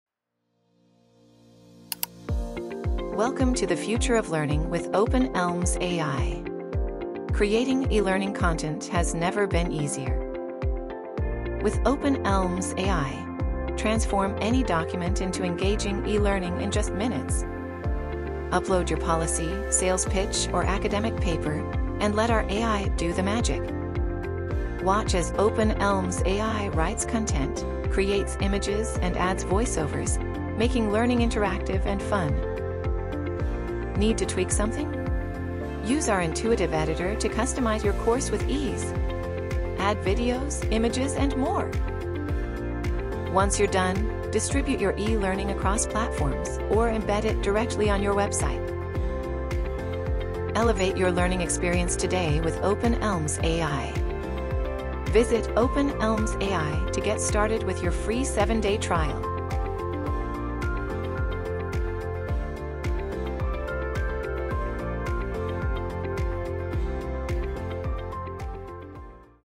Open eLMS AI: Effortless Course Creation in 60 Seconds
Welcome to the future of learning with Open eLMS AI!

Creating eLearning content has never been easier. With Open eLMS AI, transform any document into engaging eLearning in just minutes!

Upload your policy, sales pitch, or academic paper, and let our AI do the magic.

Watch as Open eLMS AI writes content, creates images, and adds voiceovers, making learning interactive and fun.

Need to tweak something? Use our intuitive editor to customize your course with ease. Add videos, images, and more!

Once you're done, distribute your eLearning across platforms or embed it directly on your website.

Elevate your learning experience today with Open eLMS AI. Visit Open eLMS AI to get started with your free 7-day trial!